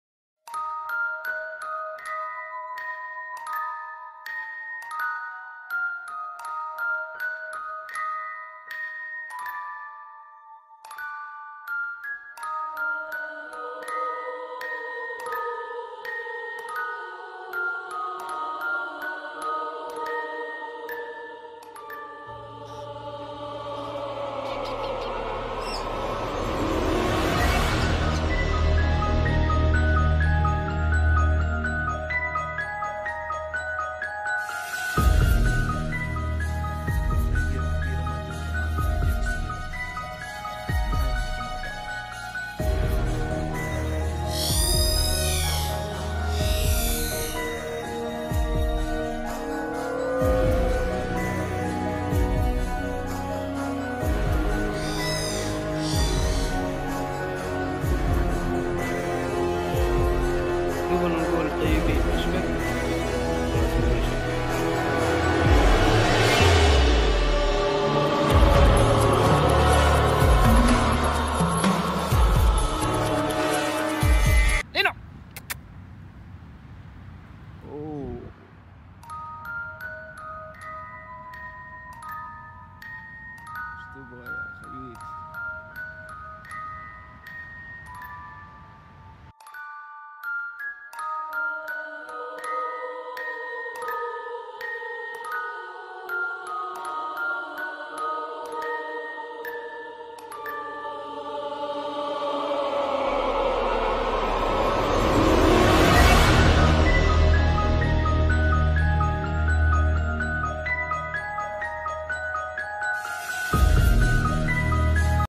كابوس الببغاوات .. parrots’ nightmare
السينغالي في طبعه غيور جدا على شركه ومكانه وممكن يكون دفاعه شرس !! هل فعلام ممكن يكون كابوس ببغاوات 💀